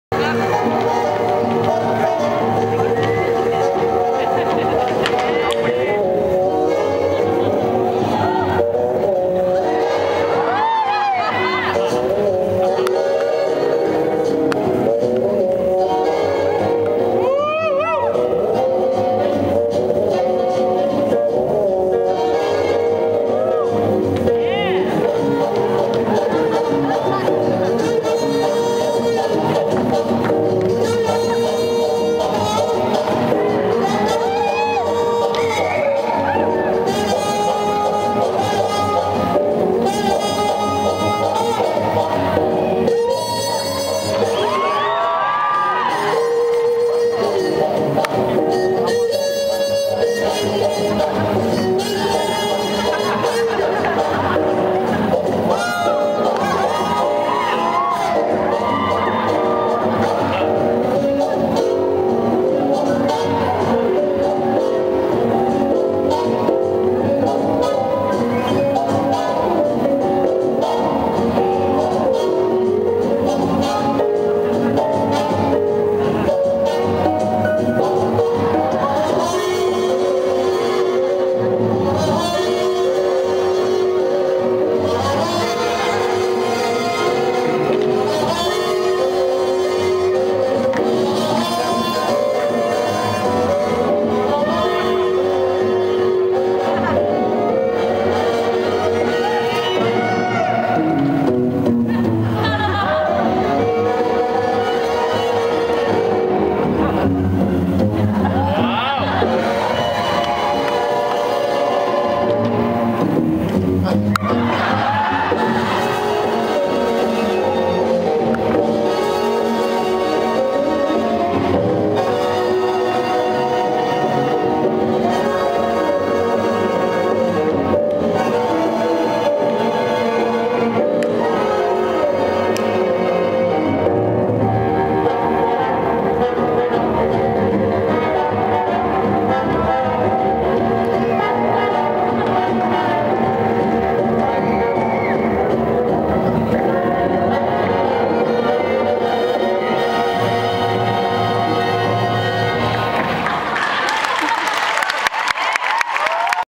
Frankie 95 - Harlem Hot Shots Mon Night Performance 2 of 2
Featuring professional dancers - Lennart Westerlund, Frida Segerdahl, Sakarias Larsson and Fatima Teffahi